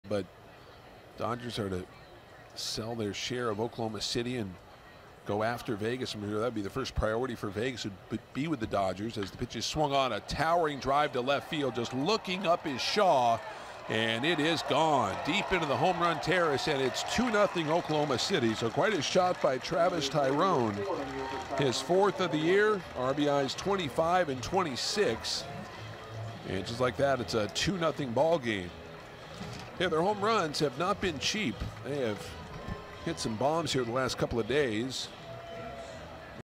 OKC's Taijeron homers
6/7/18: Travis Taijeron homers to start off his big night against Sacramento